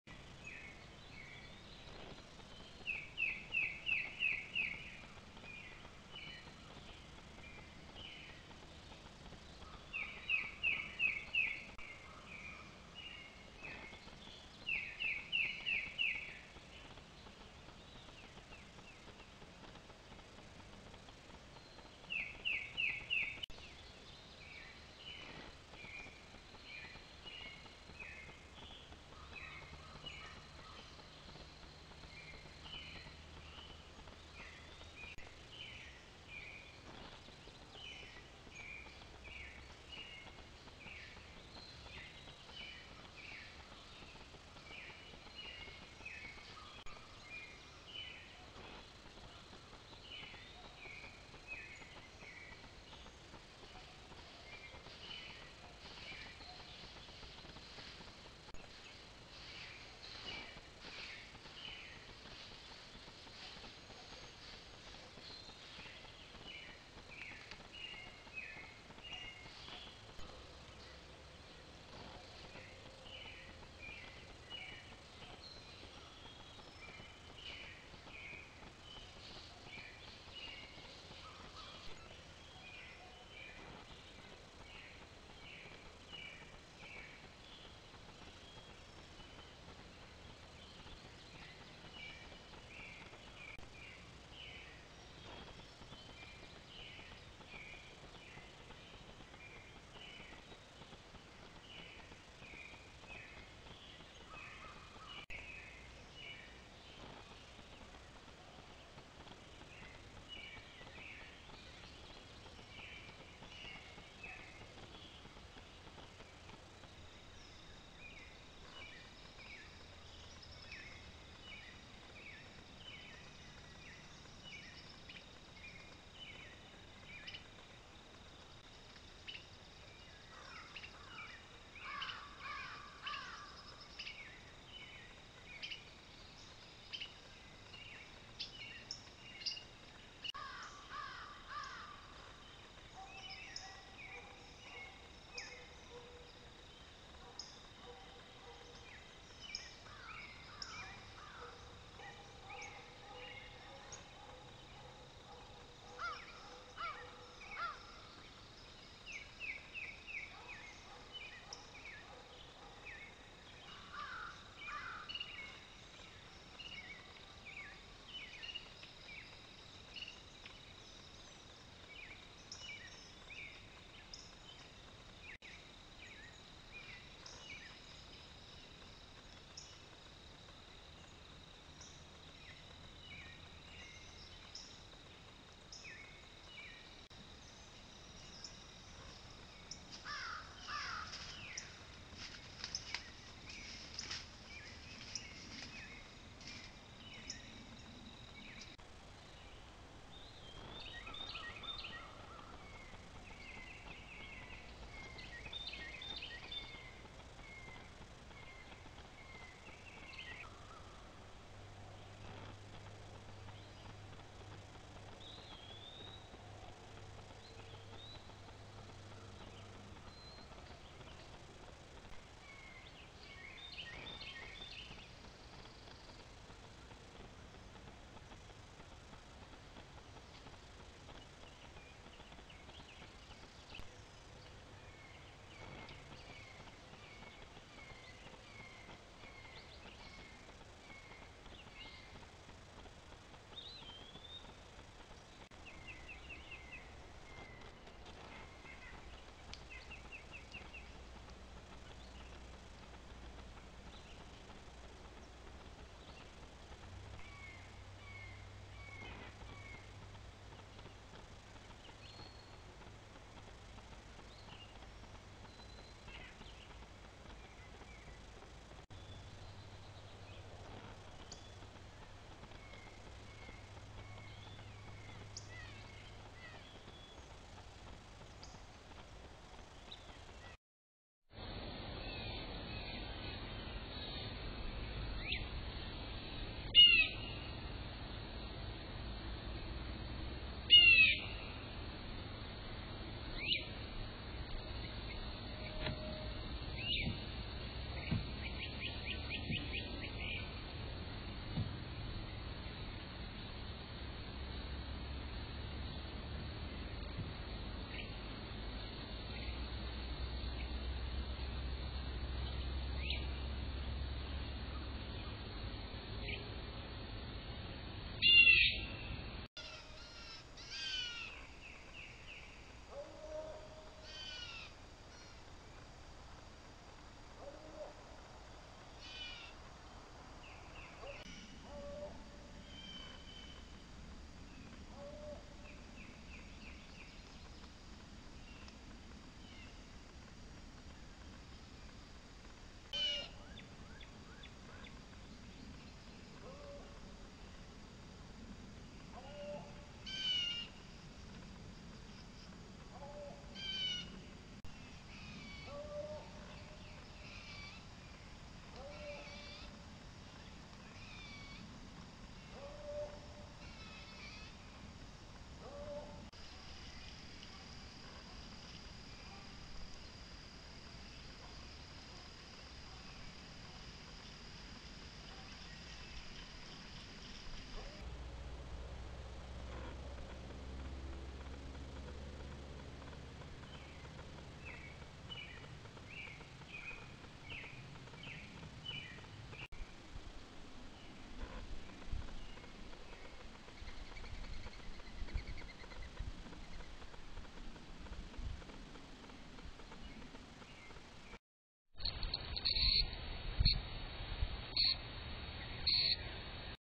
06 21 24 Daytime visitors to the farm Daily Long Video
Videos of the many animals that visited of the many animals that visited the farm.